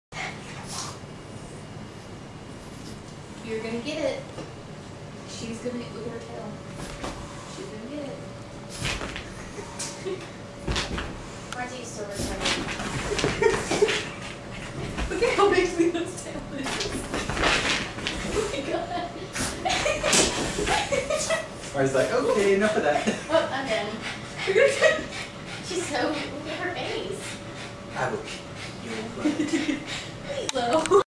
Lilo vs. Marzipan
Marzipan just wanted to get to know the kitties, but Lilo wasn't having it.